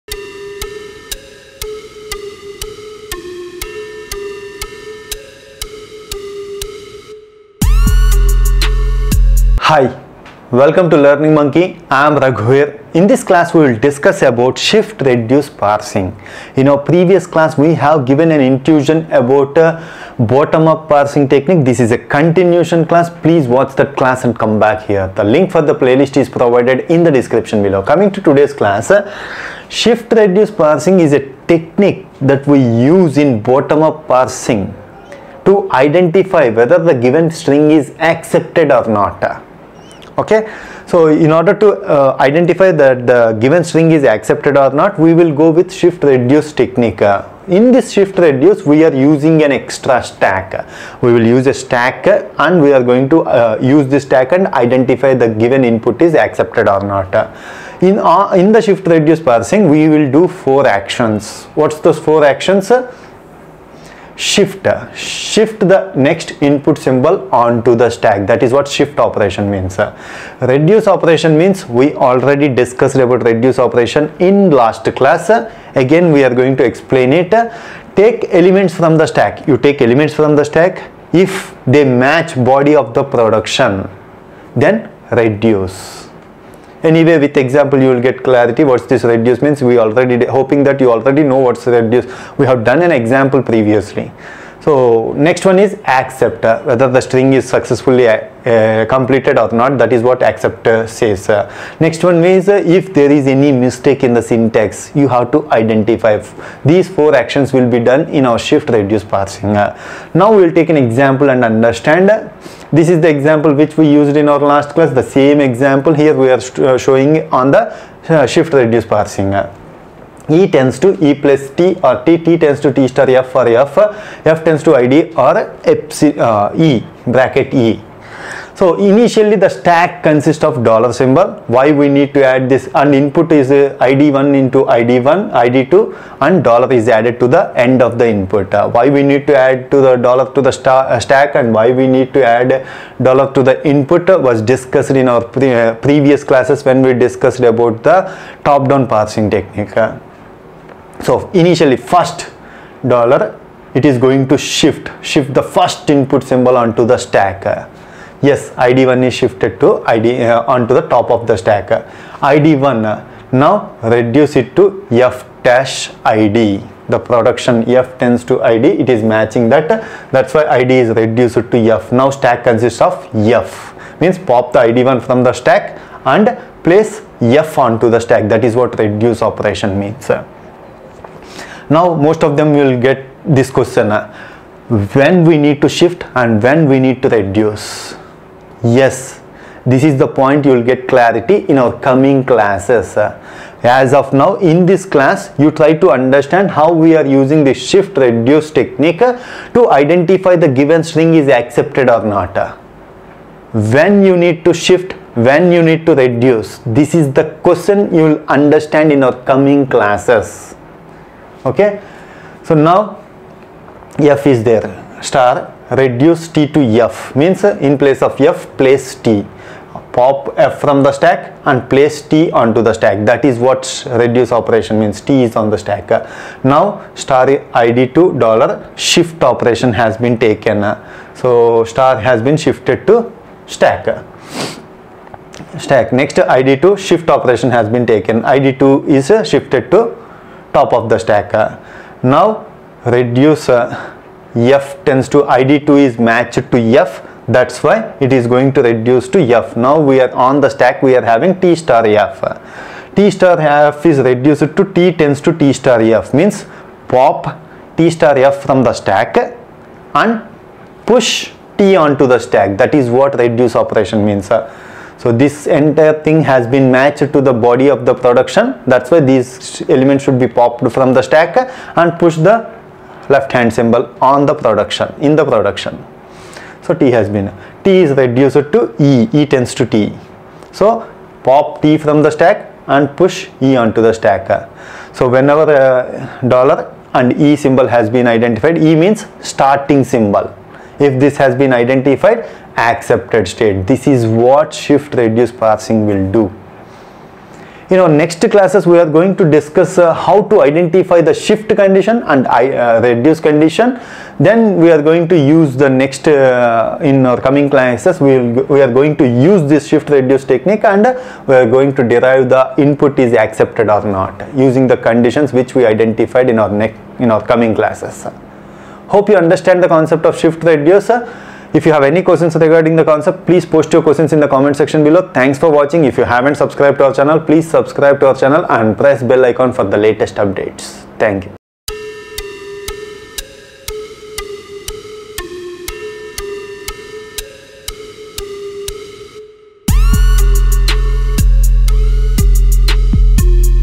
Shift Reduce Parser || Lesson 27 || Compiler Design || Learning Monkey
Shift Reduce Parser
In this class, We discuss Shift Reduce Parser.
The reader should have a prior understanding of the Bottom-up parsing example. Click Here.
Shift Reduce parsing identifies the acceptance of input strings in Bottom-up parsing.
We use an extra stack in shift-reduce parsing to parse the input string.
Actions in shift-reduce parsing.
1) Shift: Shift the next input symbol to the stack.
2) Reduce: Take elements from the stack. If they match the body of production, then reduce.
3) Accept: Successful completion
4) Error: Discover syntax mistake.
The below example shows the shift-reduce actions for the expression grammar.
Initially, the stack contains a dollar symbol.
The input string is placed in a buffer, and adding dollar at the end of the input string.
The first line shows shift action. The input id1 shifted to stack.
The second line is taking reduced action.
The id1 popped from the stack, and the non-terminal F pushed onto the stack.
The id1 matched the production F – id., so we took reduced action.
Important: When do we need to do shift and reduce?
The reader will get clarity on when to shift and reduce in our next classes.
In this class, you will understand the shift-reduce technique for accepting input strings.
Similarly, the remaining shift and reduced actions are shown in the table.
Finally, the stack remains with the starting non-terminal, and the input buffer containing the dollar is accepted state.